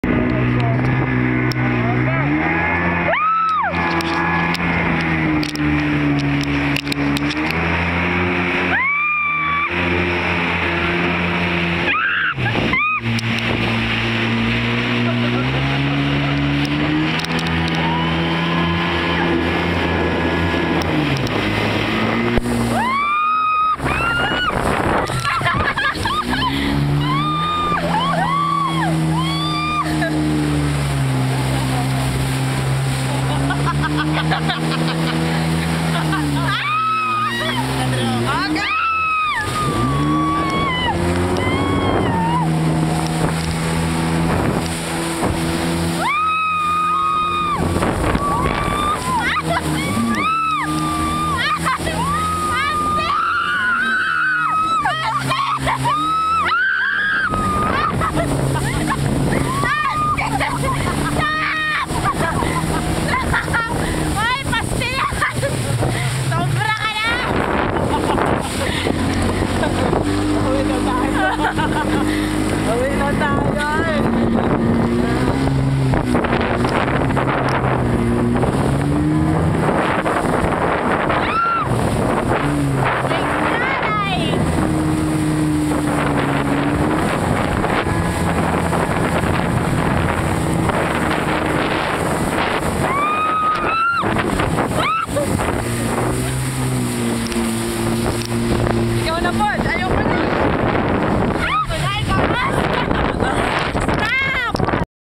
My mom's reunion-Tambuli Beach Resort Cebu 2011
Jet skiing with my aunt. Only 1,500php per 30 min! Great deal, enough or even excess time. The waters were a bit rough and fun, but I got a bit seasick after. LOL.